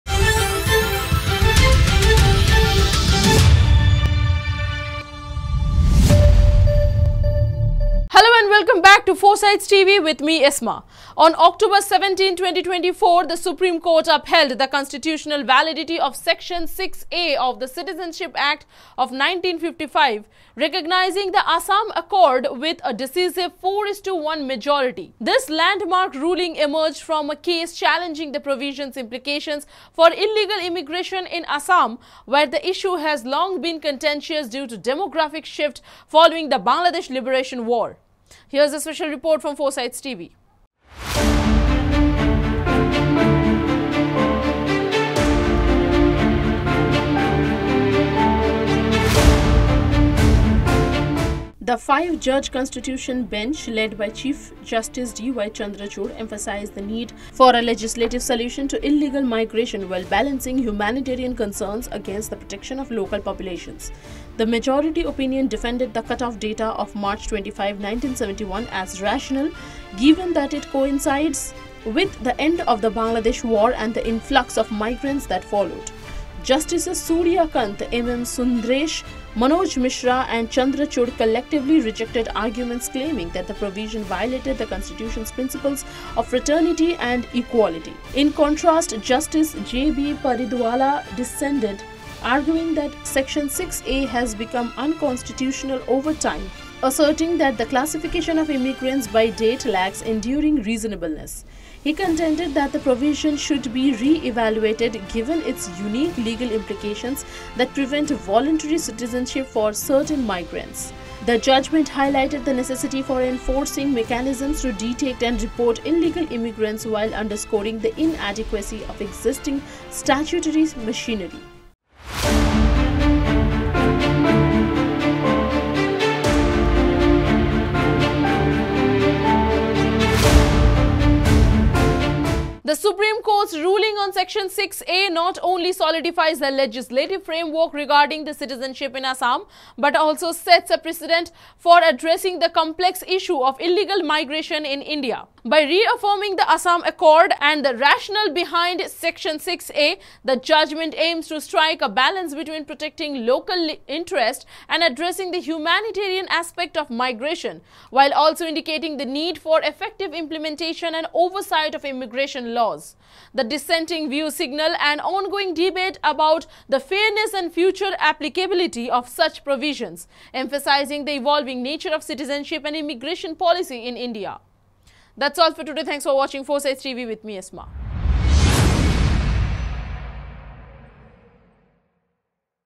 Supreme Court Upholds Section 6A Validity
On October 17, 2024, the Supreme Court upheld the constitutional validity of Section 6A of the Citizenship Act of 1955, recognizing the Assam Accord, with a decisive 4:1 majority.